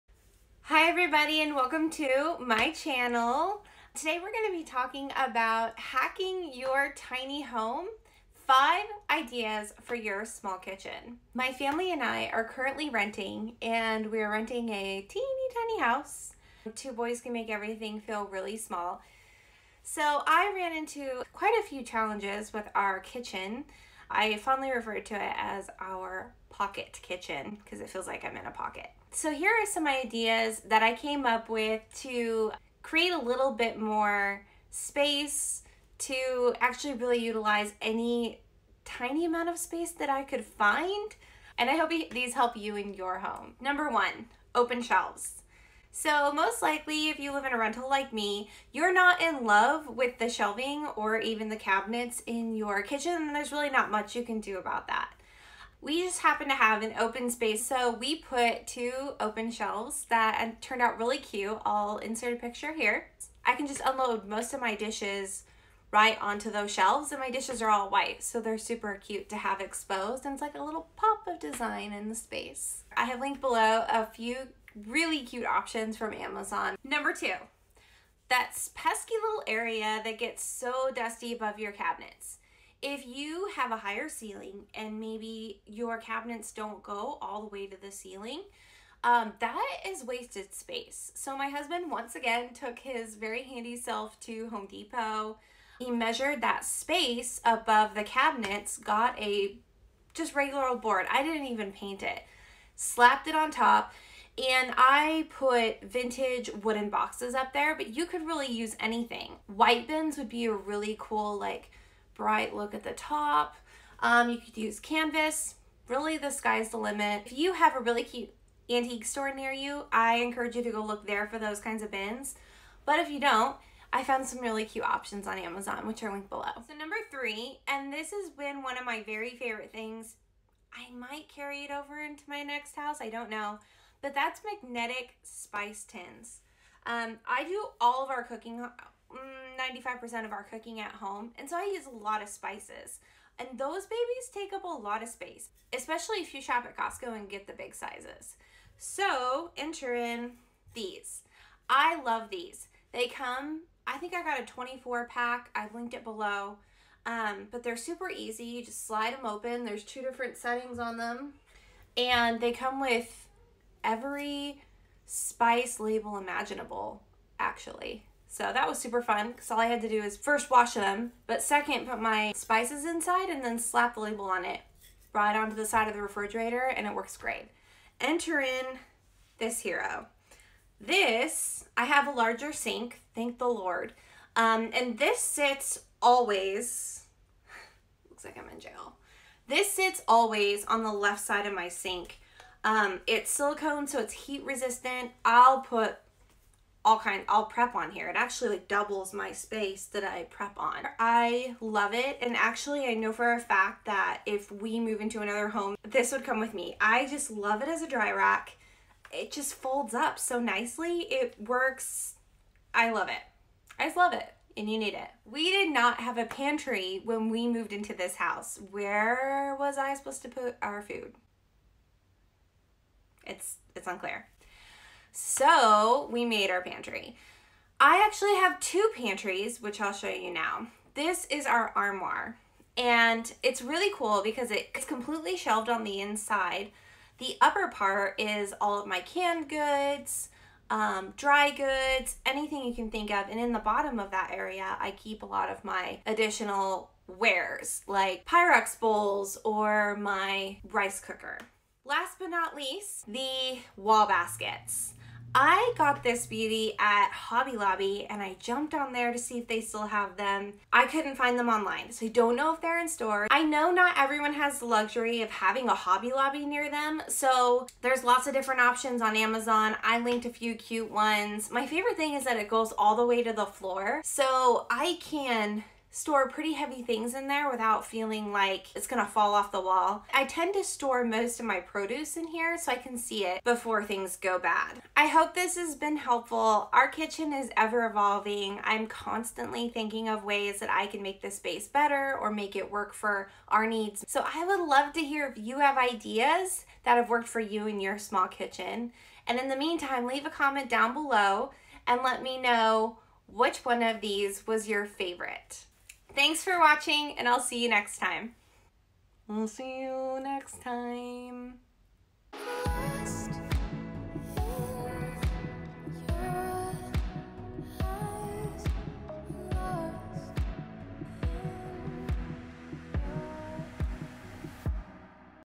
HACK YOUR TINY KITCHEN| 5 Ideas for SMALL Kitchens
Sometimes you just have to make your kitchen work, even if it's TOO SMALL. Here are 5 ideas to make it function and keep you SANE!

SHOP THIS VIDEO:
🌱Shelf option #1
🌱Shelf option #2
🌱Canvas Bins
🌱White Wooden Bins
🌱Magnetic Spice Tins
🌱Rolling Dish Rack
🌱Wire Basket Organizer